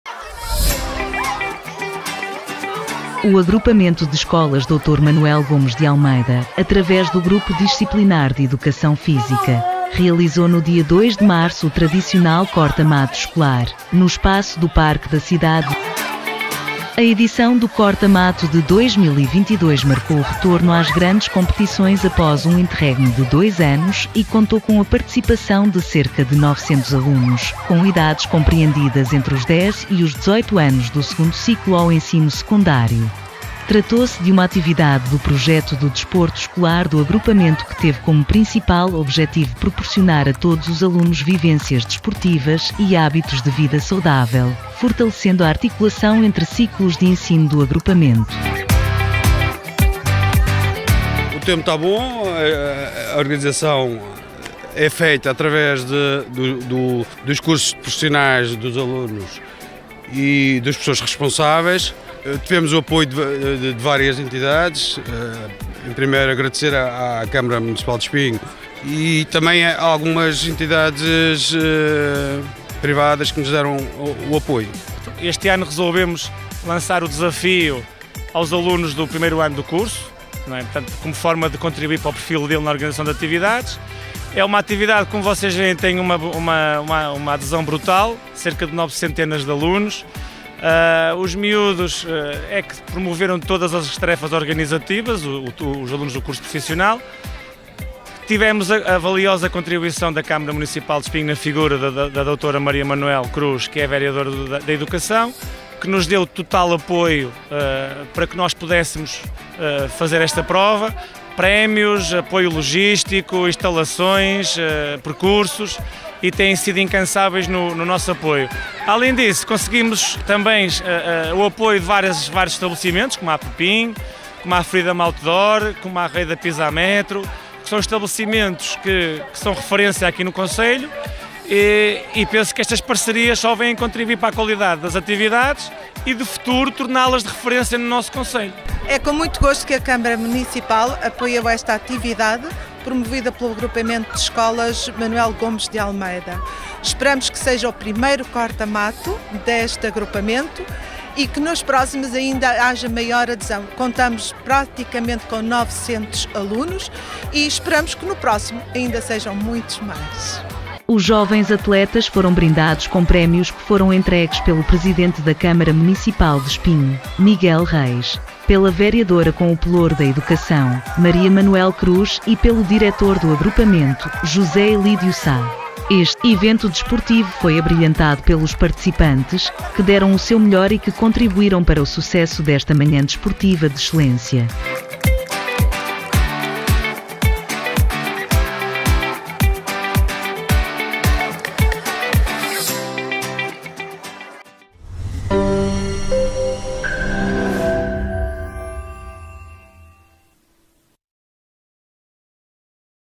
AEMGA organizou o 1º Corta-Mato Escolar Munícipio de Espinho
O agrupamento de Escolas Dr. Manuel Gomes de Almeida, através do grupo disciplinar de Educação Física, realizou no dia 2 de março o tradicional Corta-Mato Escolar, no espaço do parque da cidade de Espinho.
No ano letivo 2011/2012 formou-se o Agrupamento de Escolas Dr. Manuel Gomes de Almeida e desde então o Corta-Mato é um momento de encontro de todas as unidades orgânicas, em que os alunos competem, mas sobretudo convivem e vão construindo o sentimento de pertença ao Agrupamento.
A edição do Corta-Mato de 2022 marcou o retorno às grandes competições após um interregno de dois anos e contou com a participação de cerca de 900 alunos, com idades compreendidas entre os 10 e os 18 anos do 2.º Ciclo ao Ensino Secundário.
Tratou-se de uma atividade do projeto do desporto escolar do Agrupamento que teve como principal objetivo proporcionar a todos os alunos vivências desportivas e hábitos de vida saudável, fortalecendo a articulação entre ciclos de ensino do agrupamento.
Com vista à participação da escola no corta-mato, Fase Regional, os alunos que garantiram uma classificação entre o primeiro e o sexto lugar terão a oportunidade de conviver e competir com as centenas de alunos das escolas na Fase CAE De Entre Douro e Vouga.
Os jovens atletas foram brindados com prémios que foram entregues pelo presidente da Câmara Municipal de Espinho, Miguel Reis, pela vereadora com o pelouro da Educação, Maria Manuel Cruz e pelo diretor do agrupamento, José Ilídio Sá.
Este evento desportivo foi abrilhantado pelos participantes, que deram o seu melhor e que contribuíram para o sucesso desta manhã desportiva de excelência.
A Câmara Municipal, consciente da importância da promoção do desporto para todos, apoiou logisticamente este evento de desporto escolar. Outras entidades como os Bombeiros Voluntários do Concelho de Espinho (BVCE) e a Polícia de Segurança Pública (PSP), também marcaram presença e colaboraram nesta iniciativa.